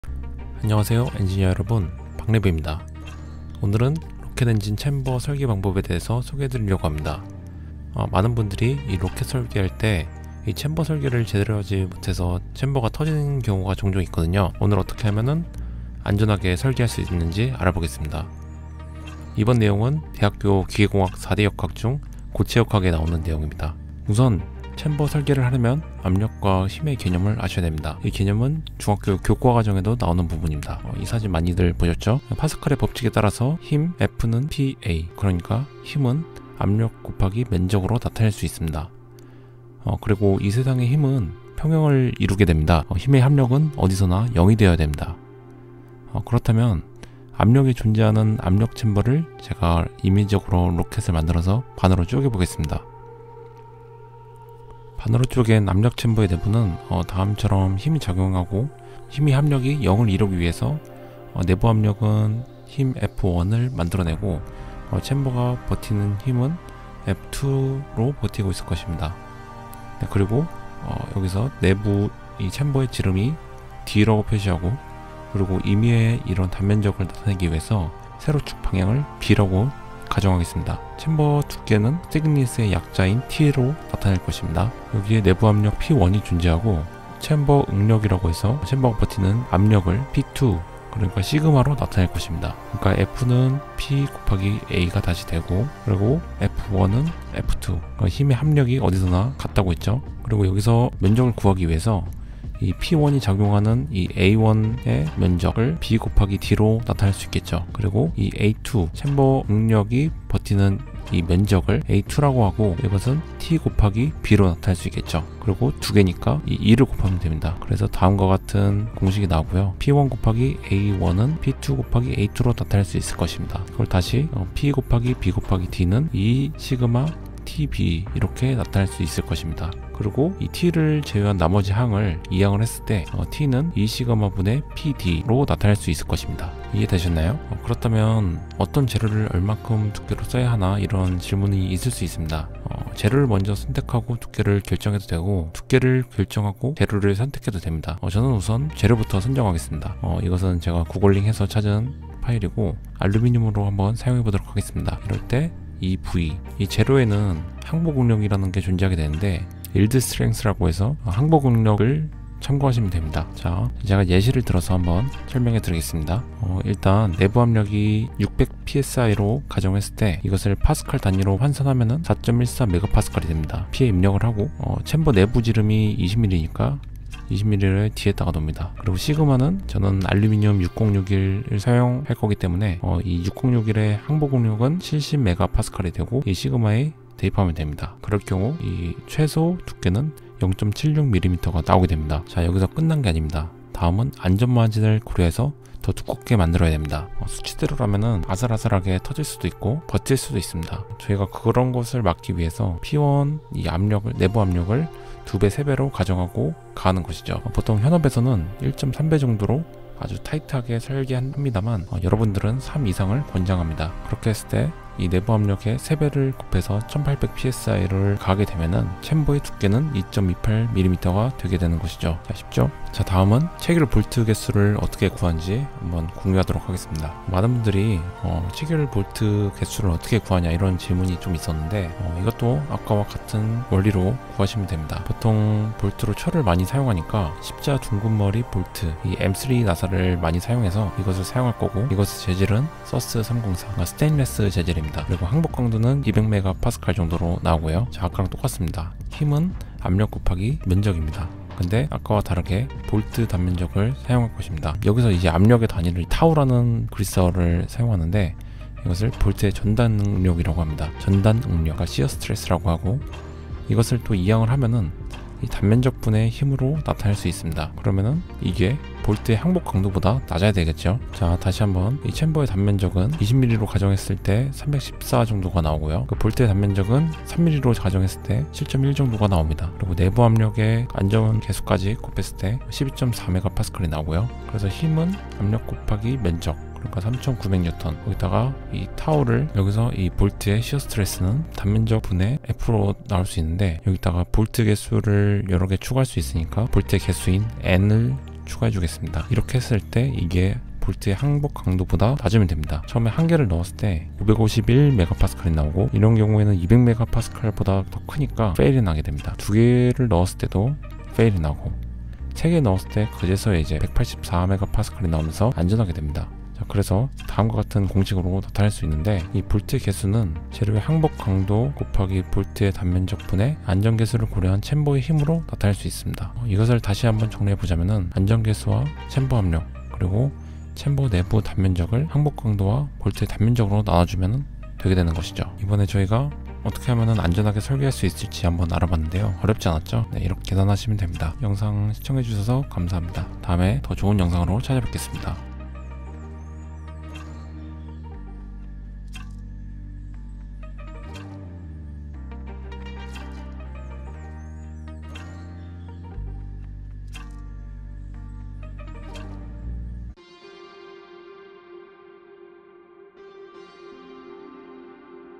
로켓 압력 챔버 안전하게 설계하기 | 자작로켓 노하우 시리즈
매주 한번씩 업로드 한다는게 찍을때마다 작품 퀄리티를 올리려고 하다보니 늦어지게 되네요.. 죄송합니다.

한 구독자분께서 압력챔버 설계 방법을 물어보셔서 이번엔 압력 챔버 설계 방법을 공유하려고 합니다.

이렇게 설계 방법을 안다면 플라스틱이든 금속이든 안전한 설계가 가능합니다.

궁금하신게 있으시다면 댓글로 남겨주시기 바랍니다.